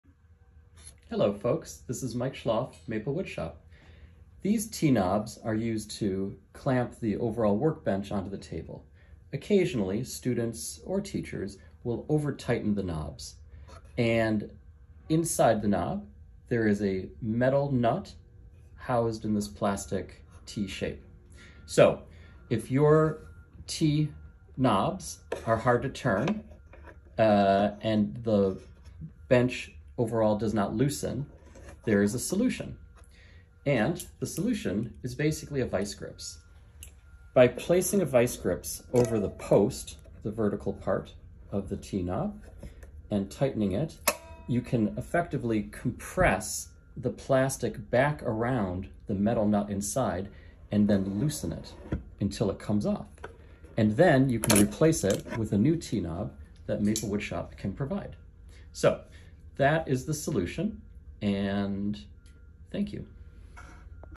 Maplewoodshop: How to replace a stripped "T" knob.
From time to time, students will overtighten the T-knobs used to hold the Maplewoodshop workbench on a table. The solution is to use a vice grip to compress the plastic onto the metal nut inside the T-knob and then loosen the T-knob. Then, you can replace the T-knob with a replacement part supplied by Maplewoodshop in the replacement parts kit.